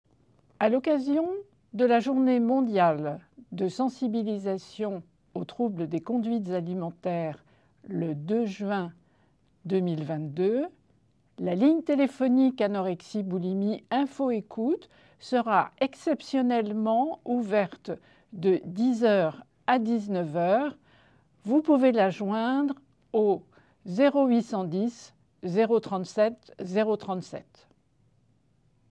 Le 2 juin 2022, la ligne ''Anorexie Boulimie Info Écoute'' sera ouverte de 10h à 19h !
Le 2 juin 2022, ouverture exceptionnelle de la ligne téléphonique TCA ''Anorexie Boulimie Info Écoute'' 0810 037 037 *, de 10h à 19h

* 0,06€ / min + prix appel